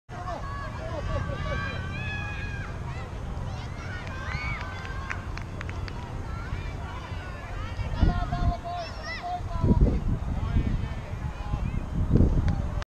Maalivahti tarjoaa syöttösuunnan
Onko maalivahti hyvin sijottunut?
Milloin maalivahdille voi syöttää?
Mitä tekee kenttäpelaajat tekee hyvin kun maalivahti saa pallon?